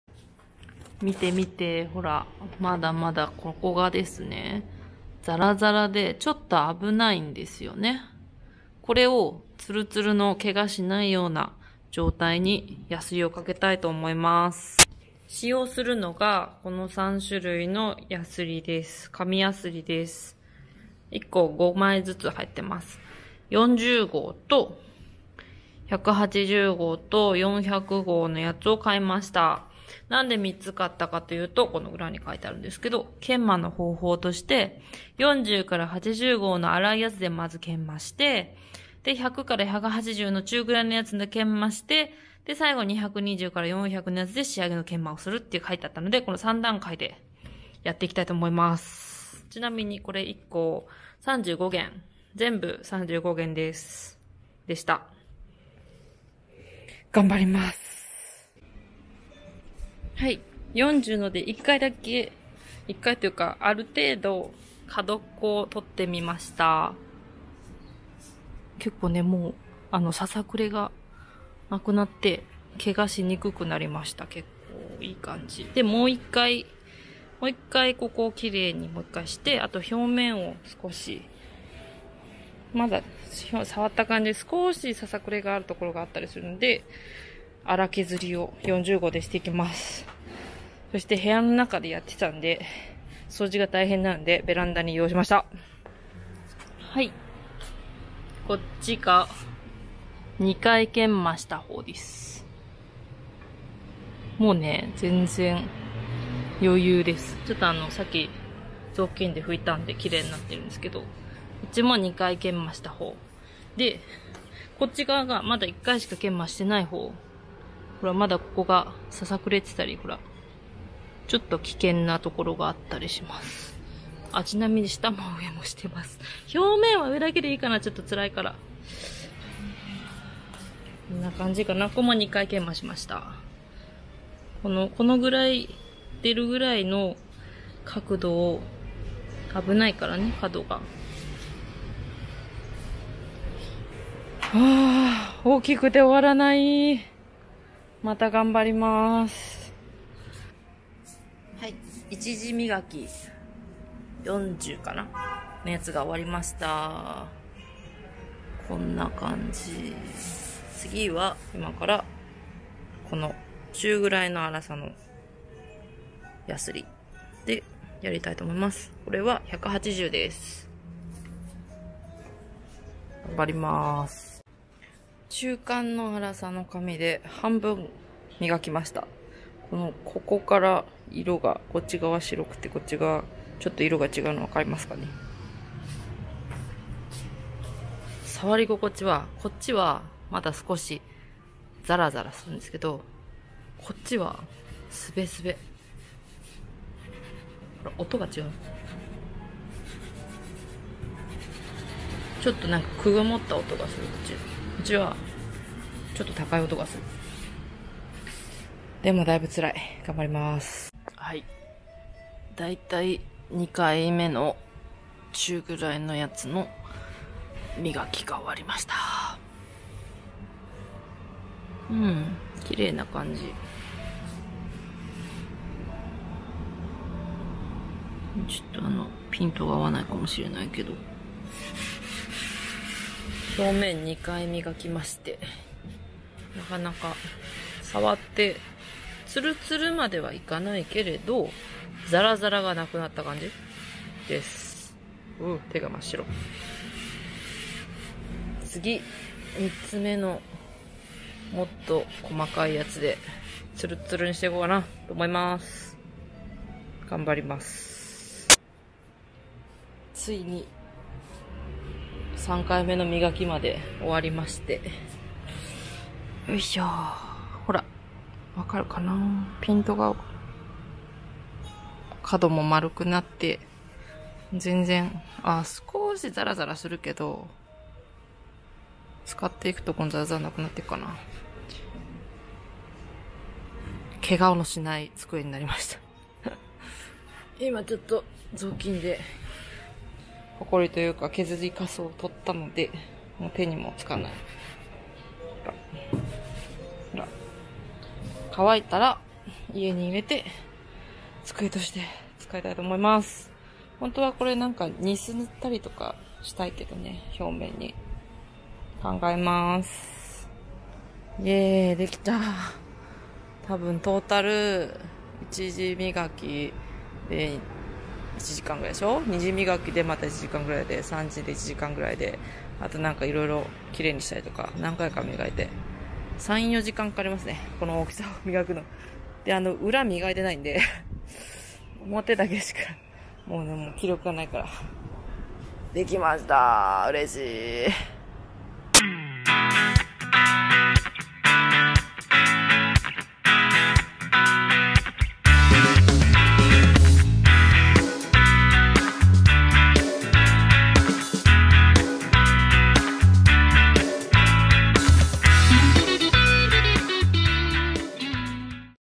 【DIY】机を紙やすりでひたすら磨いてみました
机のささくれが危なかったので、ひたすら紙やすりで磨いてみました。

機材：OSMO pocket